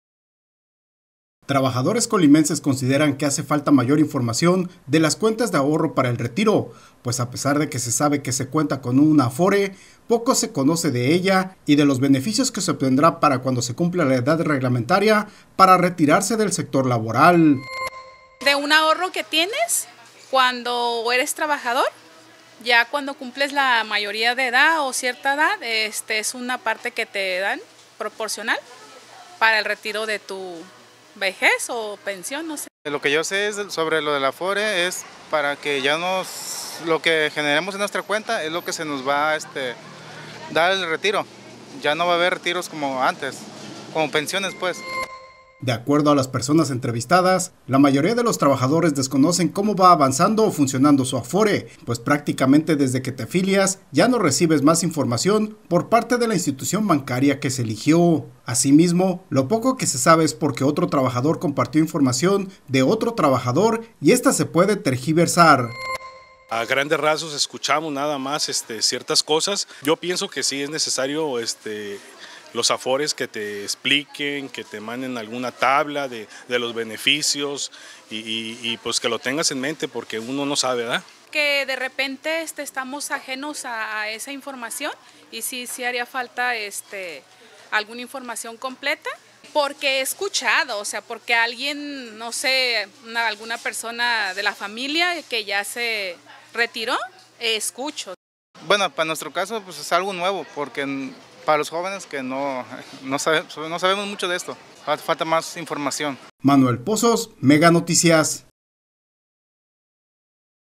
MeganoticiasColima 👨🏻‍💻👨‍🏫Hace falta más información del ahorro para retiro, opinan trabajadores
Hace falta más información del ahorro para retiro, opinan trabajadores